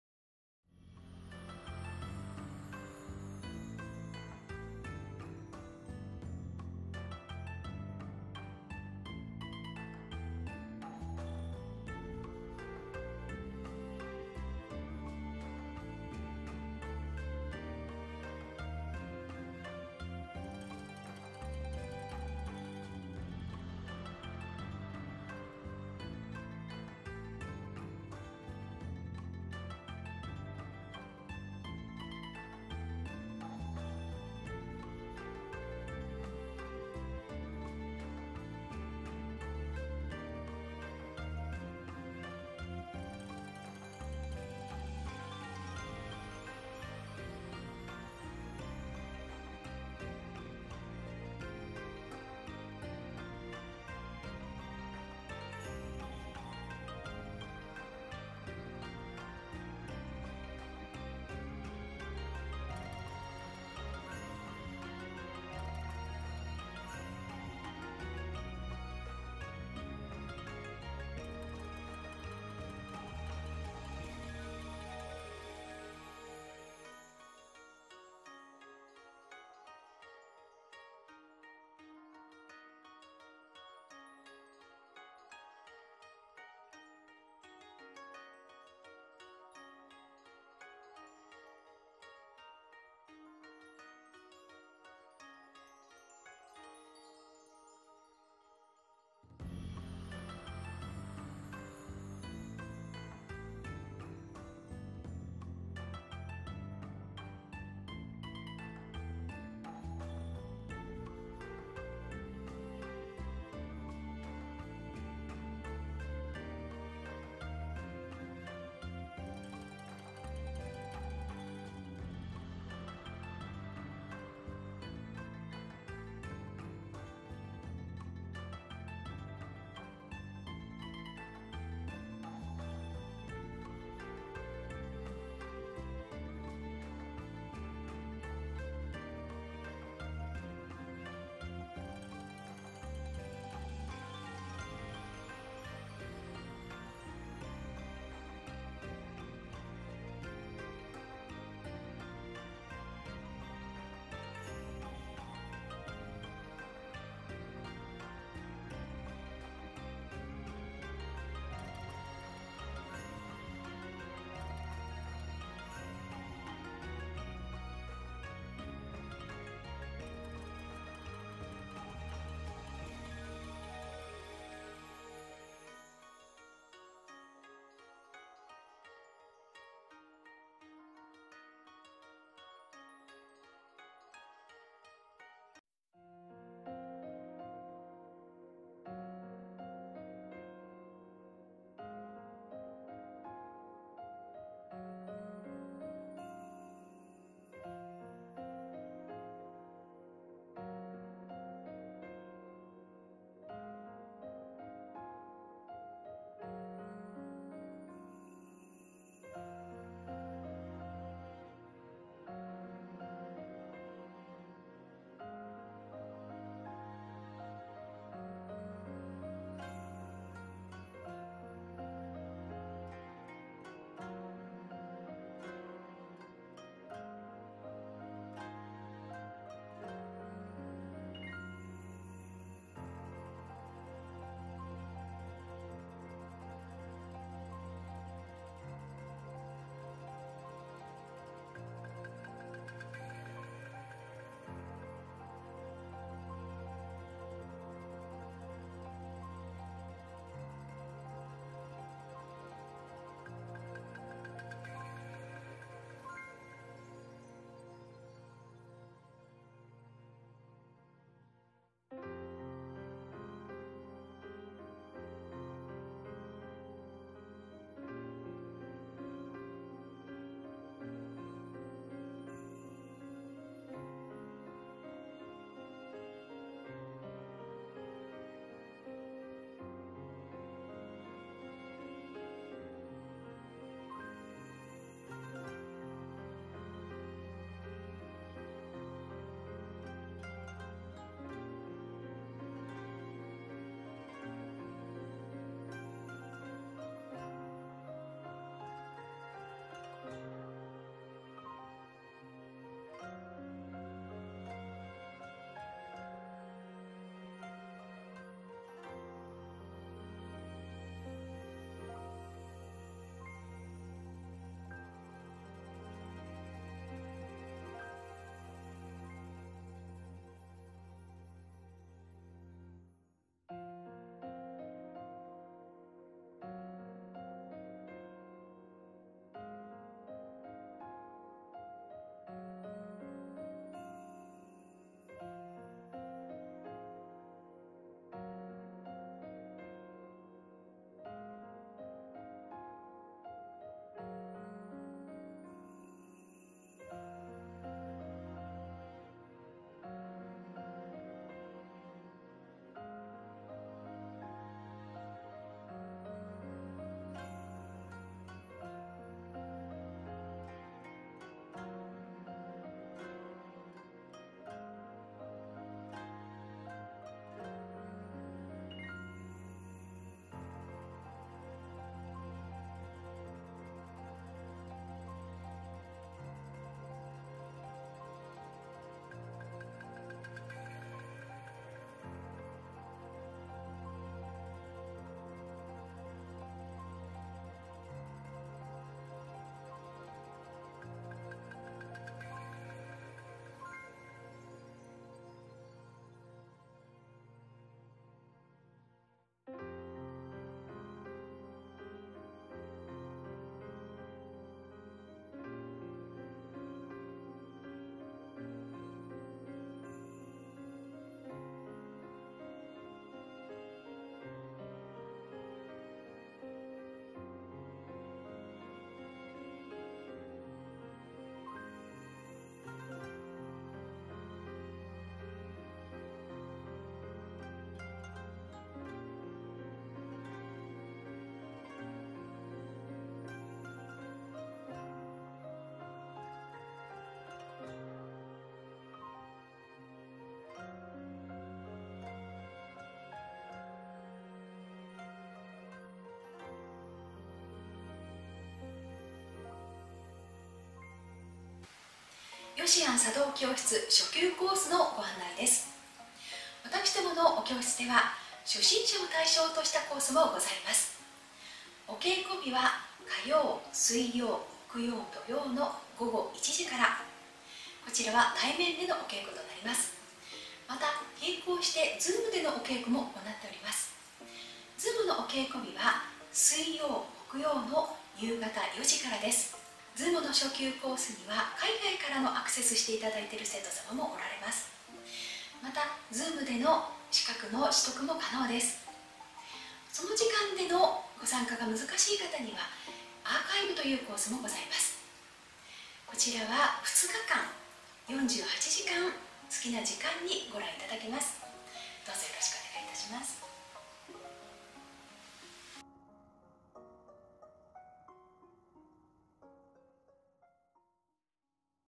『割稽古』洗い茶巾の茶巾と入子点の茶巾の扱い方の違い 神奈川県藤沢駅北口徒歩3分 茶道教室よし庵
茶道教室　よし庵